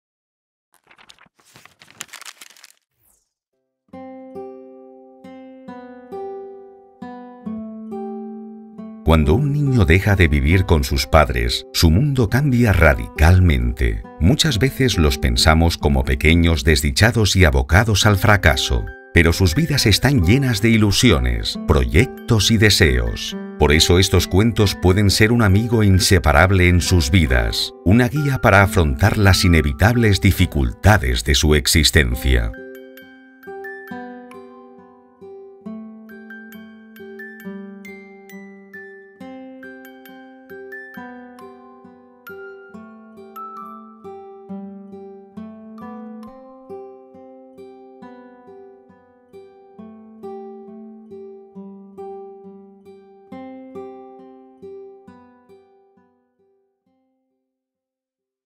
Acogimiento Familiar: Historias posibles - Letrame Grupo Editorial
Diez cuentos terapéuticos que abordan sin rodeos la realidad del Acogimiento Familiar. Acompaña a nuestros diez protagonistas, cinco niños: Jove, Maqui, Dulto, Nico, Core; y cinco niñas: Amor, Reisy, Mudy, Quía y Posi por sus historias de vida. HISTORIAS cruzadas por la violencia de género, incoherencias educativas, diagnósticos de TDAH y ansiedad, sobrecargas de rol, adultización, diferencias generacionales, indefensión aprendida, preguntas sin respuestas sobre su situación…. Pero también VIDAS llenas de ilusiones, proyectos, creencias, deseos y verdades que les pueden ayudar a salir adelante, si reciben el apoyo aunque sea de una persona que crea en ellos.